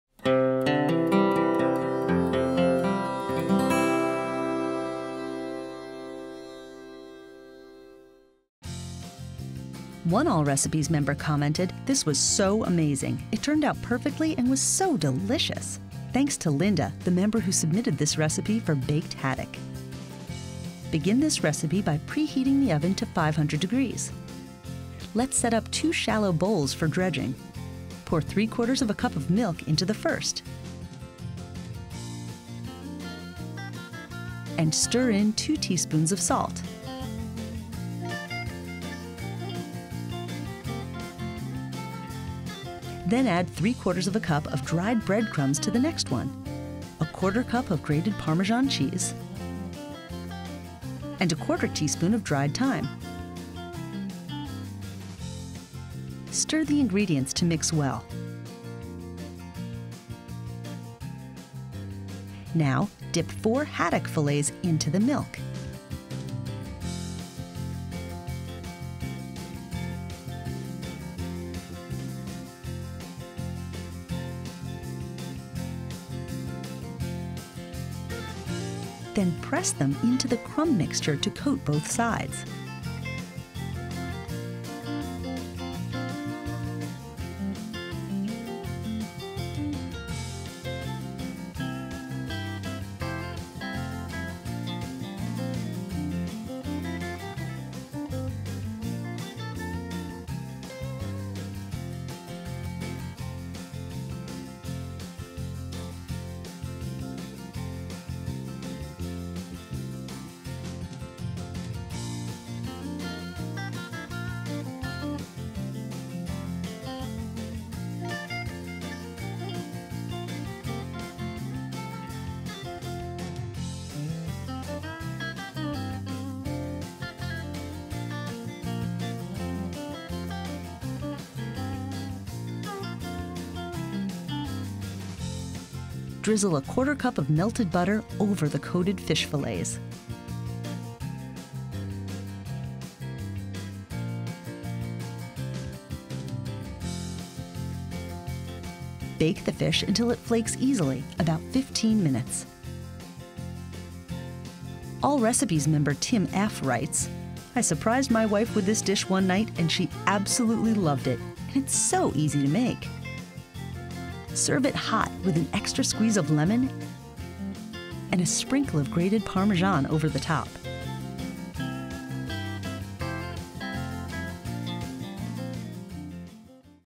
How to Make Baked Haddock | Fish Recipe | Allrecipes.com
Parmesan cheese flavors the light breading of these delicate fish fillets, while a healthy drizzle of butter ensures delectable flavor! Want to see how easy it is?